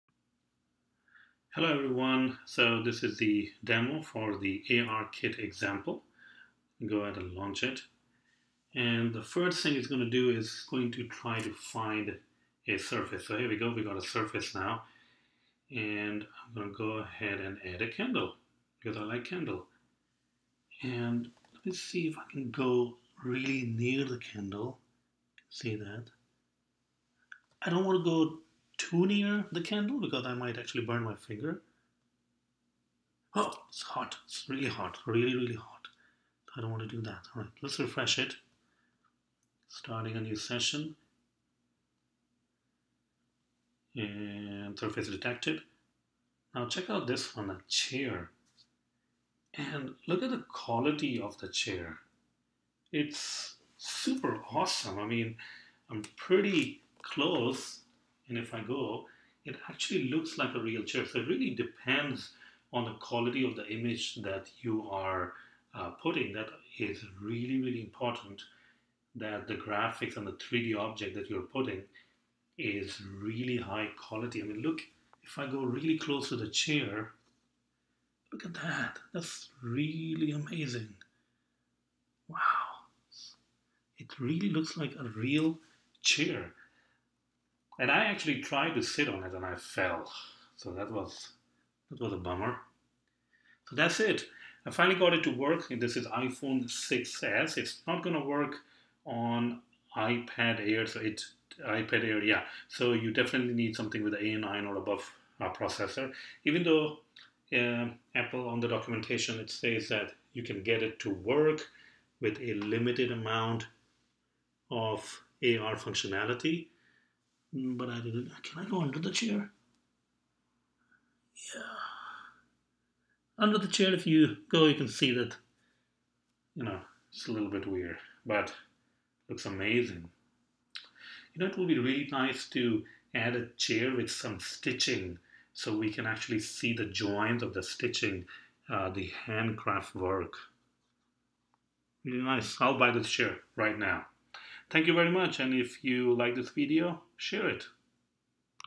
ARKit in iOS 11
Learn to create Augmented Reality Apps using ARKit for iOS.